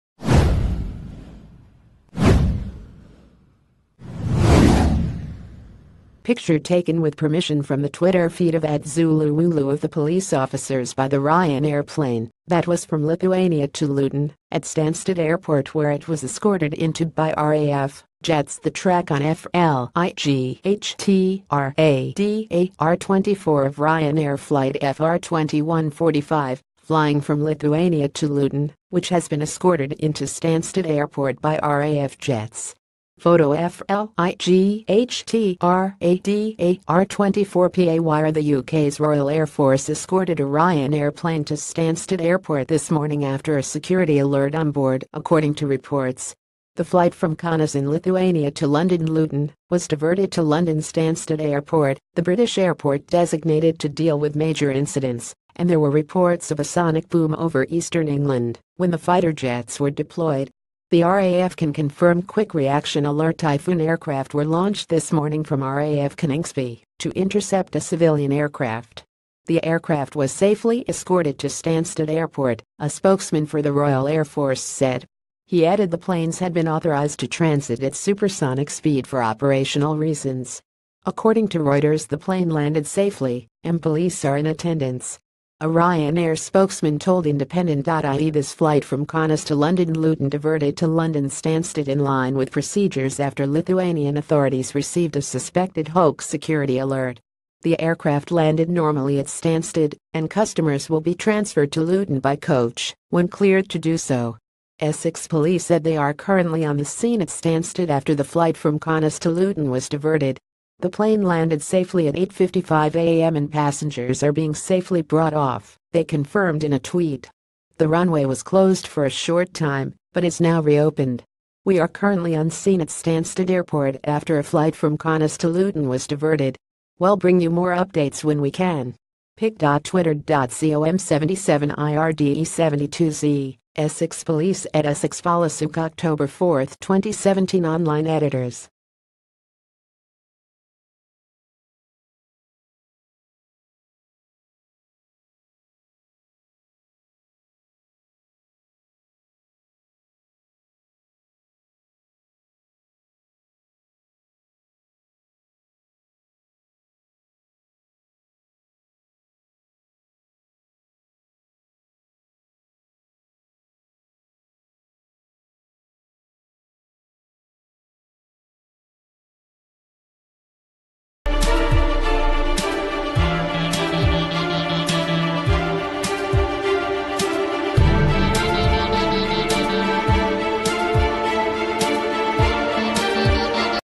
Breaking News | Raf jets escort ryanair flight to london airport after 'hoax bomb call'
Raf jets escort ryanair flight to london airport after 'hoax bomb call'

Picture taken with permission from the twitter feed of @zulu_wooloo of the police officers by the Ry...